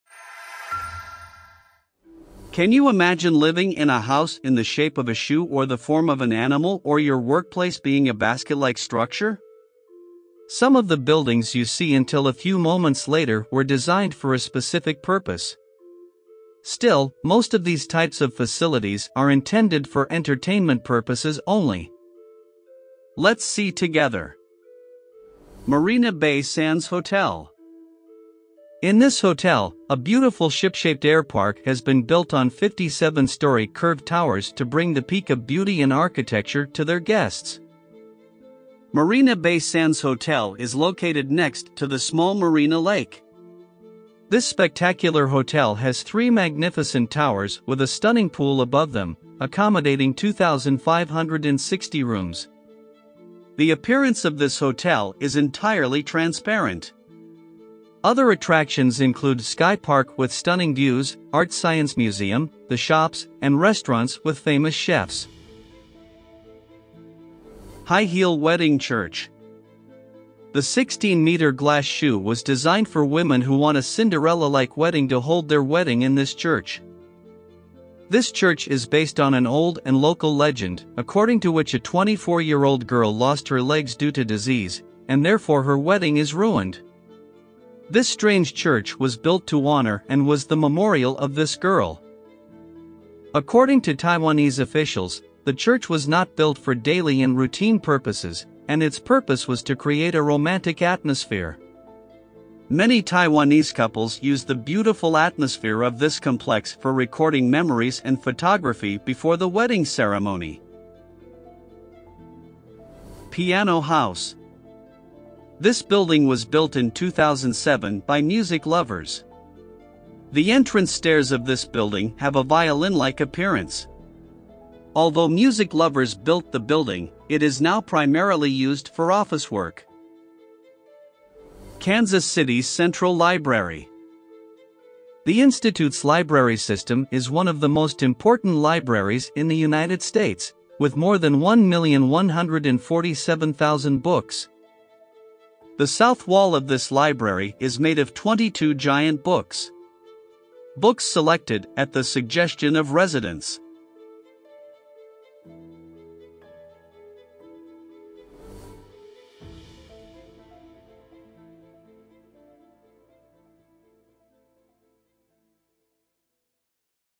Quirky Architecture: Exploring Unique and Unconventional Structures
a ship-shaped hotel soaring above the skyline to a high-heel wedding church straight out of a fairy tale, these architectural wonders redefine the boundaries of creativity and imagination. Join us as we explore the Marina Bay Sands Hotel in Singapore, the High-Heel Wedding Church in Taiwan, the Piano House in China, and the Kansas City Central Library in the United States.

Discover the fascinating stories behind these extraordinary structures and marvel at their breathtaking designs. Whether you're a lover of architecture or simply curious about the world's most unusual buildings, this video is sure to leave you inspired!